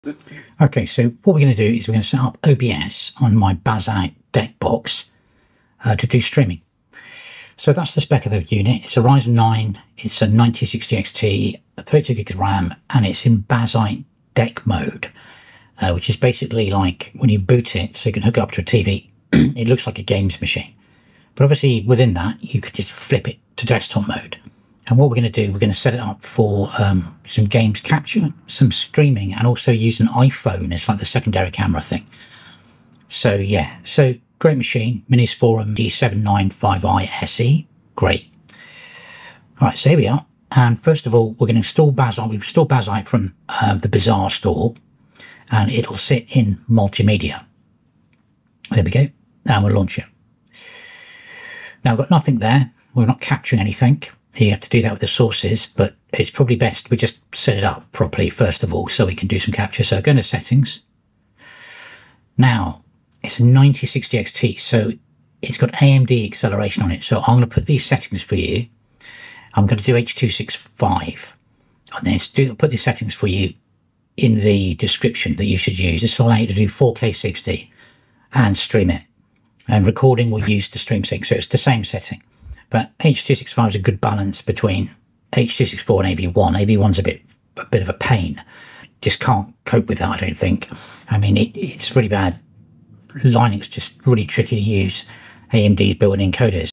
This Tiny Titan Destroys OBS Streaming: Ryzen 9 & Radeon Power Unlocked!
Witness the process of transforming a Minisforum BD795i SE into a dedicated streaming station. The walkthrough details the precise OBS configuration needed to harness the Ryzen 9 and Radeon 7060 XT's power, specifically locking in H265 encoding for balanced 4K 60fps performance using AMD hardware acceleration. An ingenious workflow is then demonstrated for adding an iPhone as a reliable, low-latency secondary camera feed via DroidCam OBS over a direct USB-C connection. All the essential setup steps for screen capture and camera integration on this Linux powerhouse are laid bare.
00:00